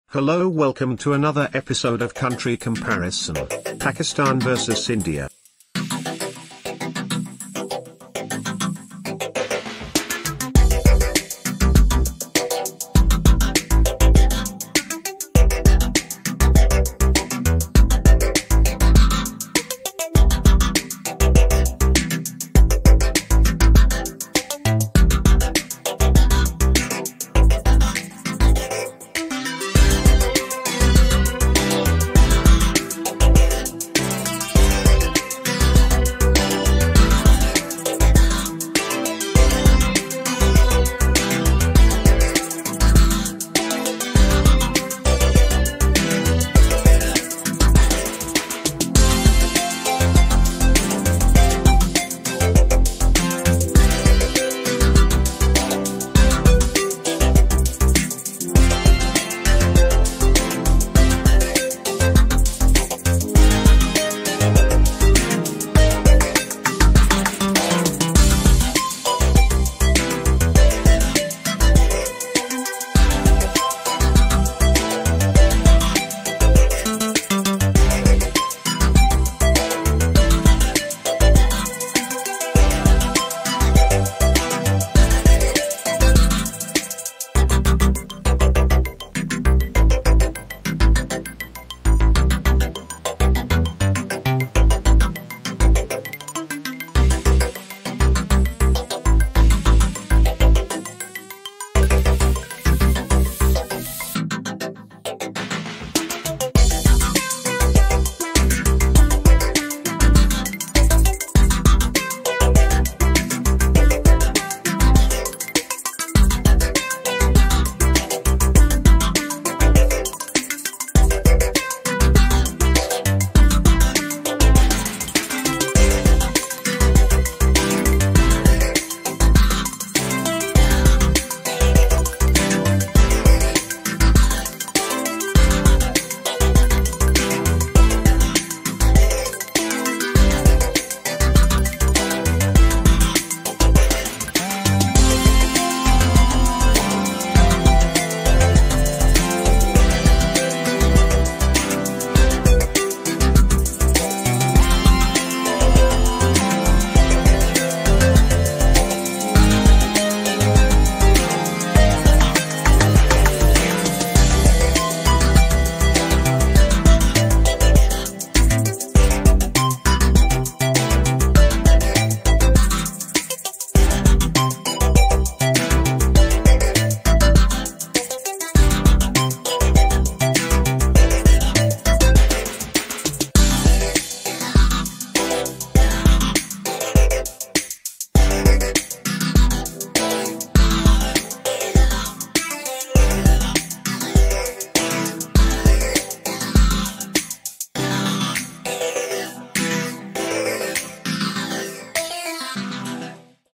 Pakistan vs India - Country Comparison 2024
Land Area, Government, Economy, Military, Gdp, Export, Import, Olympic Medals, Nobel Prizes, National Animal, Best Dishes, Largest Company, Productions, Tourism, Internet Users, etc.

Which Country do you Think is More Influential? Share Your Views in the comment section down below.

Please review the contents. Your opinion matters Thank you for your support.